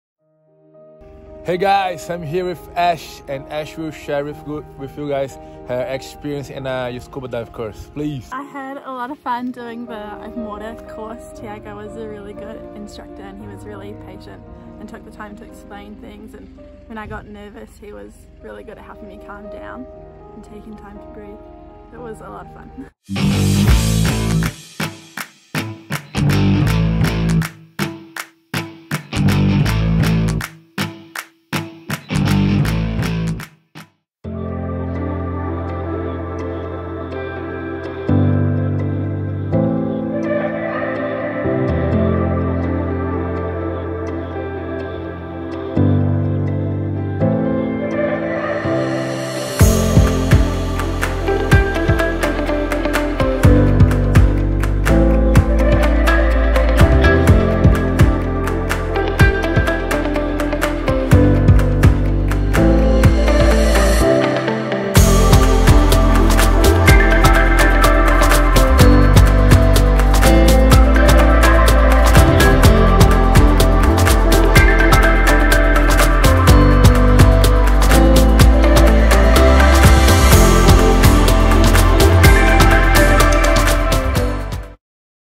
Open Water Scuba Dive Course of Ashleigh Jane Reed - New Zealand
After challenging herself, finally Ash conquered her license.